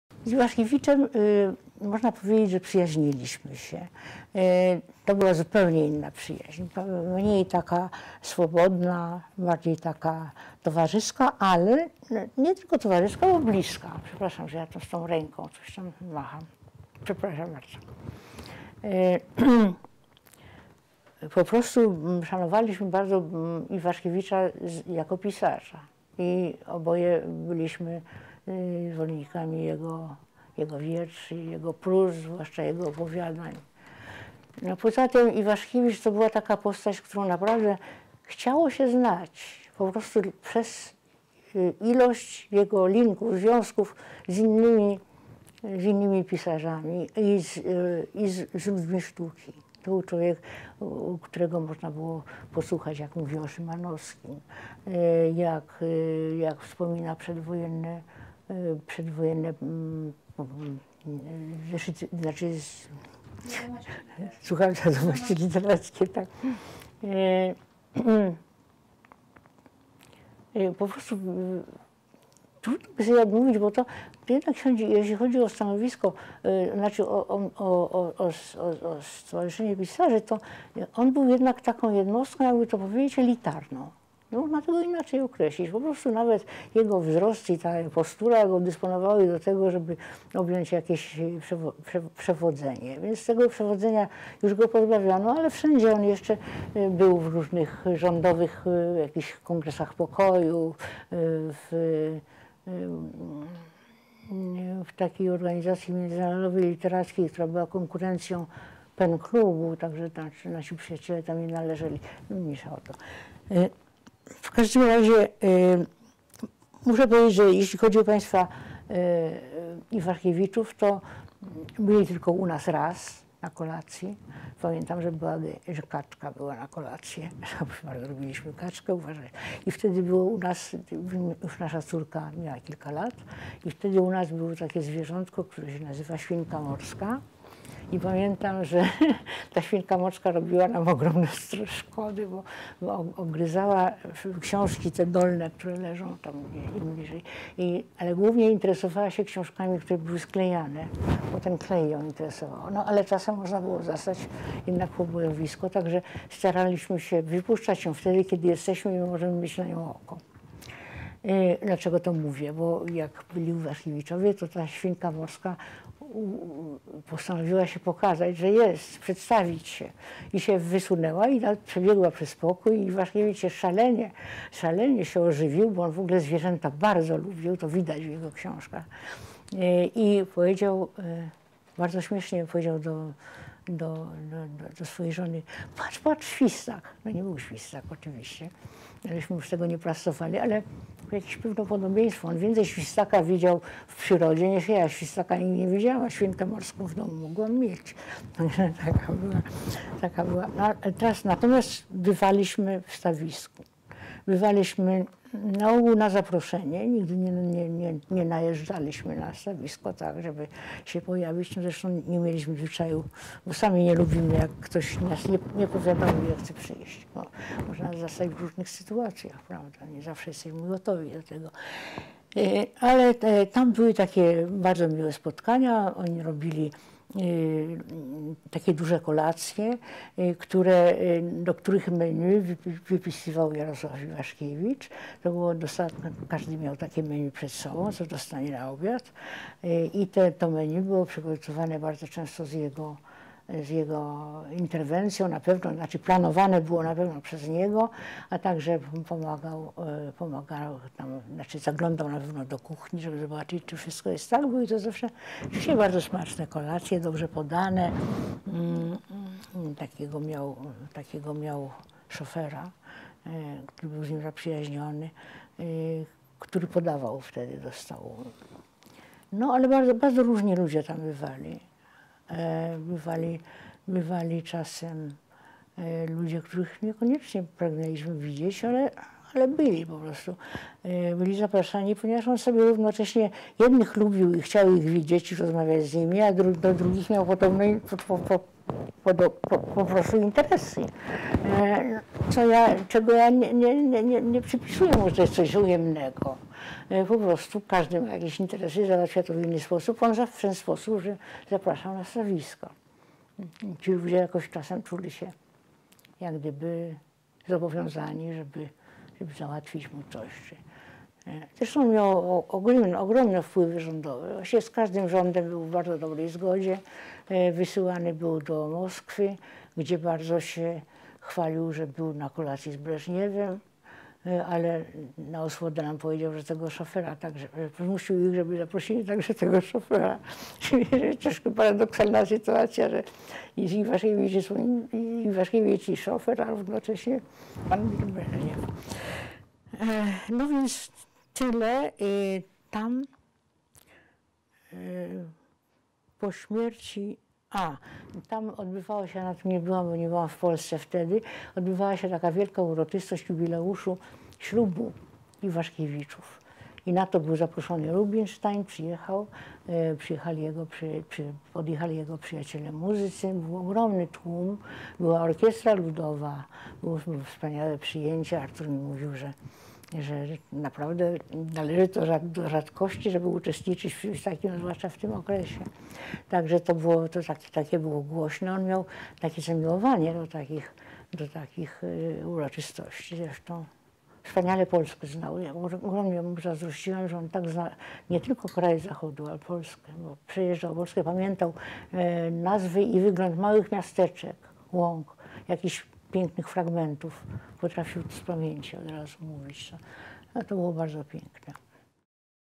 Julia Hartwig - Our admiration of Iwaszkiewicz (62/100)
Julia Hartwig (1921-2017) was a Polish poet, essayist, translator and author of children's books. She studied at universities in Warsaw, Lublin and Kraków. Czesław Miłosz called her 'the grande dame of Polish poetry'. [Listener: Andrzej Wolski; date recorded: 2010]

TRANSCRIPT: You could say that we were friends with Iwaszkiewicz. It was a completely different friendship, less casual, more comradely but it wasn't just comradely because we were close – I apologise for waving my hand around like this, I'm very sorry. We respected him greatly as an author and we were both fans of his poetry, his prose, especially of his short stories. In addition, Iwaszkiewicz was the sort of person you just wanted to know simply because of the number of connections with other, with other writers and with people involved with the arts. You could listen to him talking about Szymanowski or about the pre-war... pre-war Zeszyty, I mean...

[AW] Wiadomości Literackie?

Pardon?

[AW] Wiadomości Literackie.

Wiadomości Literackie, yes. It was hard to deny yourself because in terms of his status, I mean in the Writers' Association, he was a unique, you could say elite, individual. You can't really describe him in any other way, even his height and his posture predisposed him to take... take charge. He had been relieved of his role as leader by then but he was still in various government peace congresses, in an international literary organisation which competed with the PEN Club so, yes, our friends didn't belong to it, but that's not important.

I have to say that as far as Mr and Mrs Iwaszkiewicz were concerned, they only came to us once for dinner. I remember that we served duck, we were always very partial to duck... we had – our daughter was only a few years old – we had a little pet called a guinea pig. And I remember that this guinea pig used to do a lot of damage because it would nibble the books that were on the bottom, lying lower down but mainly it focused on books whose spines had been glued because it liked the glue. Occasionally, we'd find a total mess, so we tried to only let it out when we were there to keep an eye on it. Why am I telling you this? Because when the Iwaszkiewiczes were there, the guinea pig decided to show itself, to show that it was there, to introduce itself and so it peeked out and then ran across the room. Iwaszkiewicz became extremely animated because he loved animals anyway – you can see that in his books – and said, it was very funny, he turned to his wife and said, 'Look, look, a marmot'. It wasn't a marmot, of course, but we didn't disabuse him. They do look alike, he's seen more marmots in their natural environment than I have, I've never seen a marmot but I had a guinea in my home, this was... this was...

However, we did go to Stawisko, we were invited to go there, we never... we did arrive unexpectedly at Stawisko, we didn't have that habit because we ourselves don't like it if someone doesn't tell us they're coming but want to visit, they might encounter us in all manner of situations, we might not always be prepared for a visit. We had some very nice get togethers, they used to hold these big dinner parties with menus written out by Jarosław Iwaszkiewicz, it was... everyone had one of these menus telling them what they were going to be served for dinner. And these menus were very often prepared with his involvement, I mean, they were planned by him and I'm sure he also kept an eye on the kitchen to make sure that everything was as it should be. These dinners were always delicious and very beautifully presented. He had a... he had a chauffeur who was his friend and he used to serve at table. Very different people used to come there.

Sometimes, there were people there whom we didn't necessarily want to see but they were... they had been invited because some people he liked and wanted to see them and talk with them, whereas with others he apparently had deals he wanted to sort out, which I don't... I'm not saying there was anything wrong in this. Everyone has deals they want to make and they make them in their own way and this was his way: he'd invite people to Stawisko. These people seemed at times to feel obligated to arrange things for him. He did, after all, have enormous influence in the government. He had very good relations with every government. He was sent to Moscow where he boasted, he had had dinner with Brezhnev, but to soften the blow he told us that his chauffeur also – he'd forced them to invite his chauffeur, too. It's slightly paradoxical that here's Iwaszkiewicz and Słon... Iwaszkiewicz and his chauffeur and there's Mr Brezhnev. So that's it.